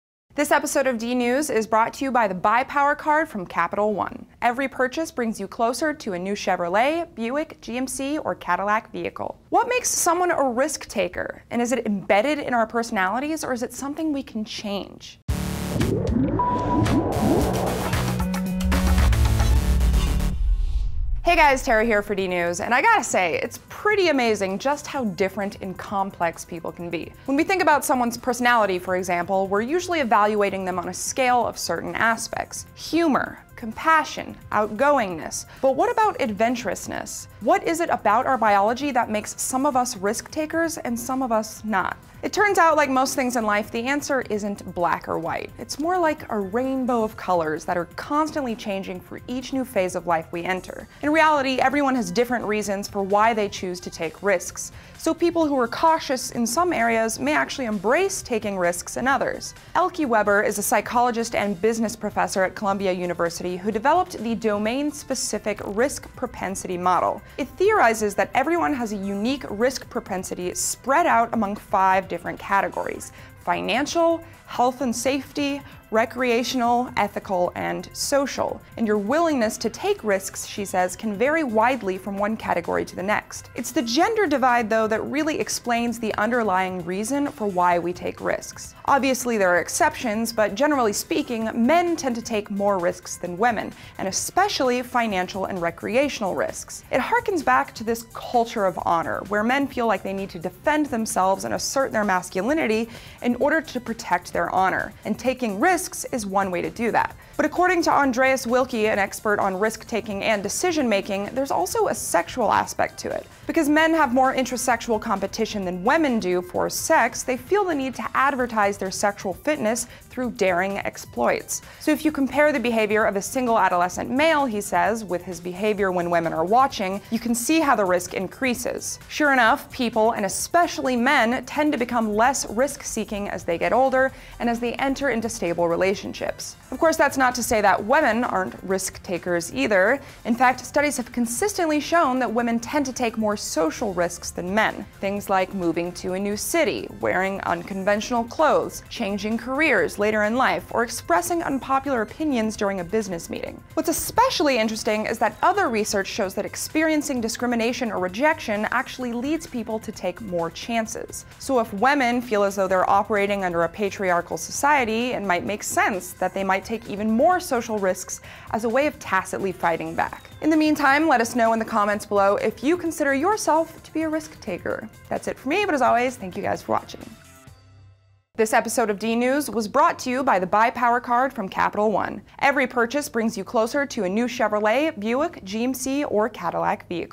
What Makes Someone A Risk-Taker?
Some people are likely to take risks, and some people aren’t. What’s the difference between these types of people? Join Tara as she explores the biology of risk-taking.

What Makes a Risk-Taker
“Paul Cusma manages his clients' money cautiously, so the retirees among them ‘don't have to go back to work,’ says the Tampa, Fla., financial adviser. In the investment realm, ‘I'm always planning for a rainy day,’ he says.”